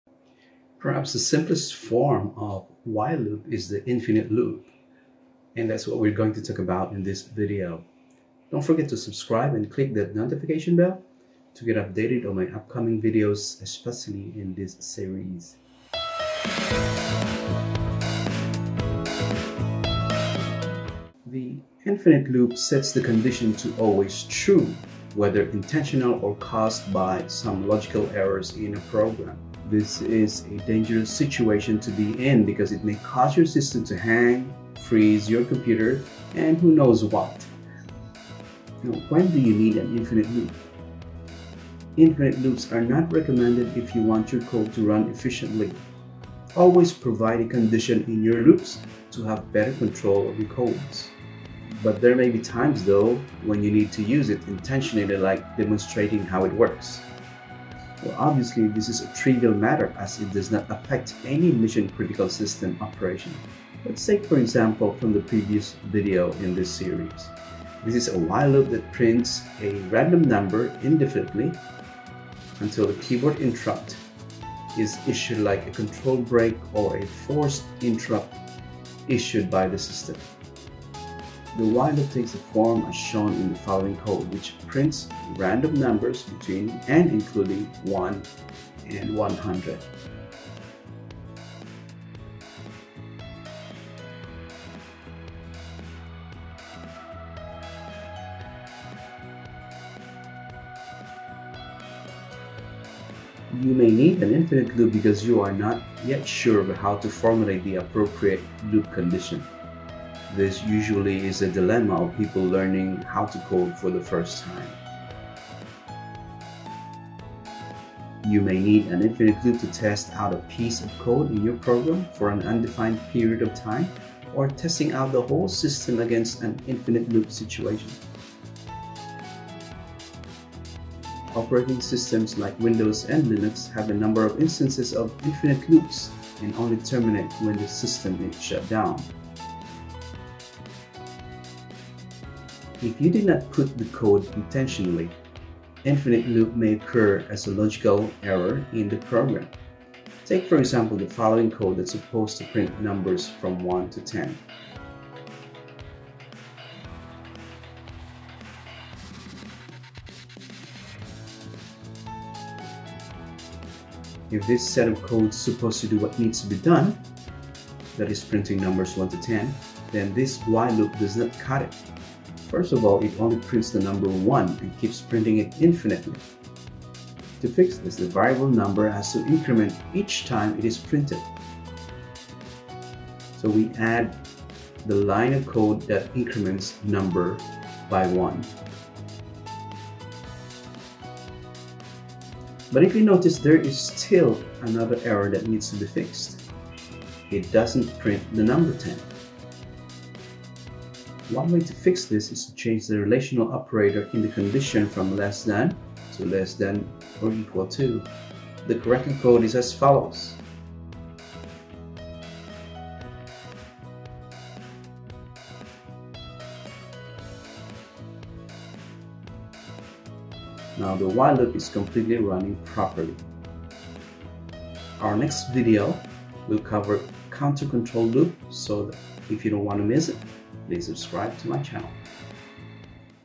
Python for Beginners in 3 Minutes | Python Infinite Loops
Perhaps the simplest form of while loop is the infinite loop. And that's what we're going to talk about in this video.